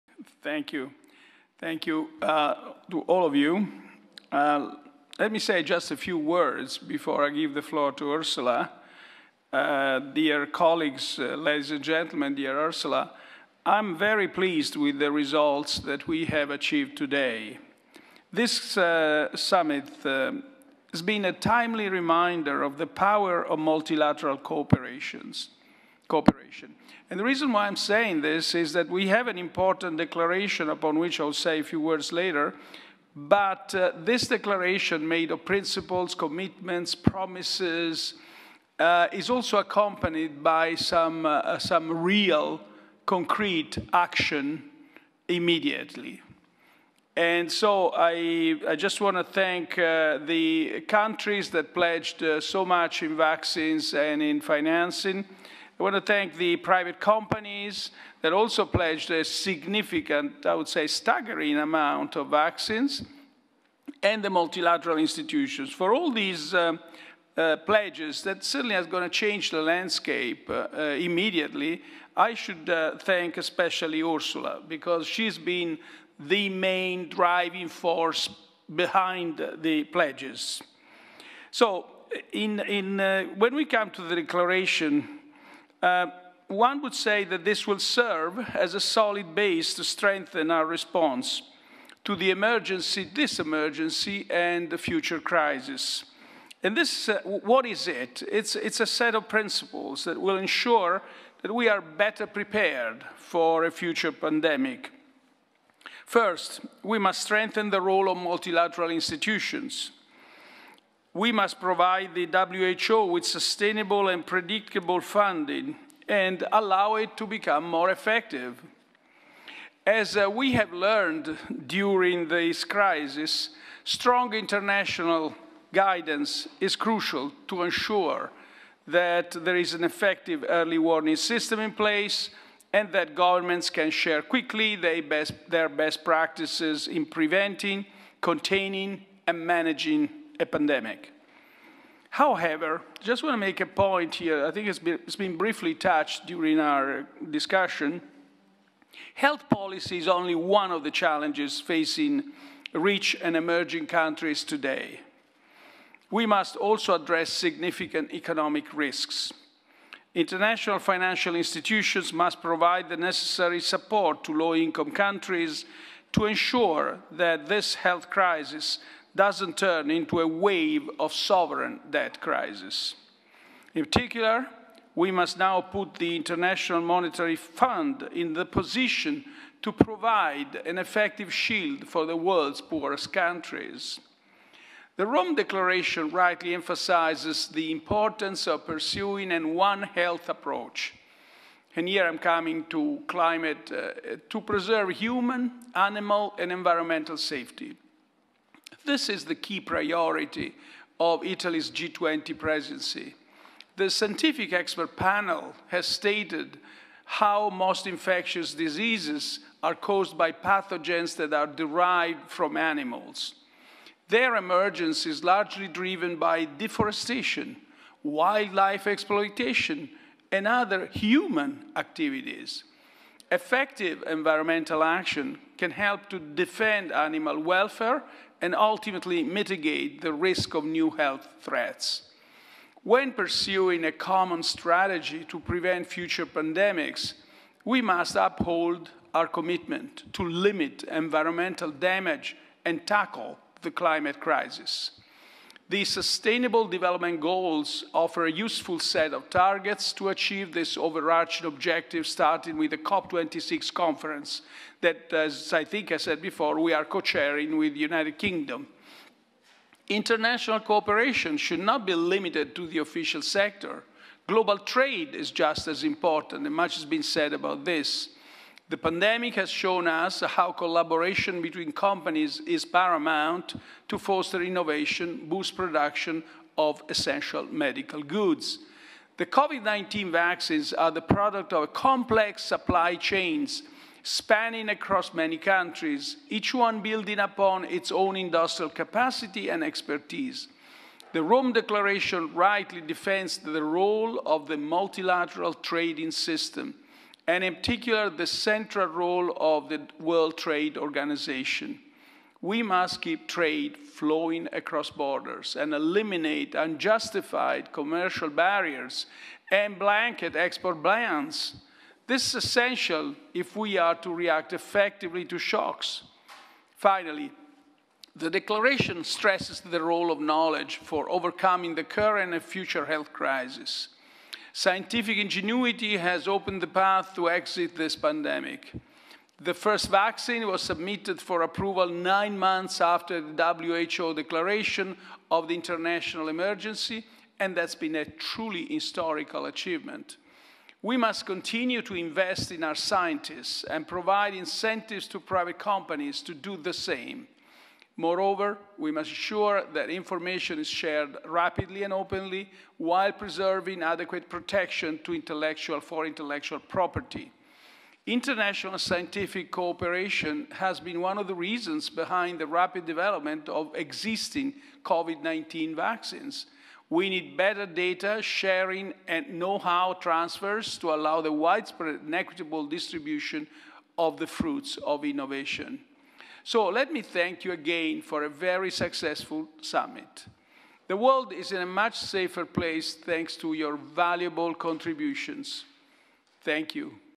GlobalHealthSummit, Closing address by Prime Minister Mario Draghi
21 May 2021, Rome - Closing address by Prime Minister Mario Draghi at Pre-Summit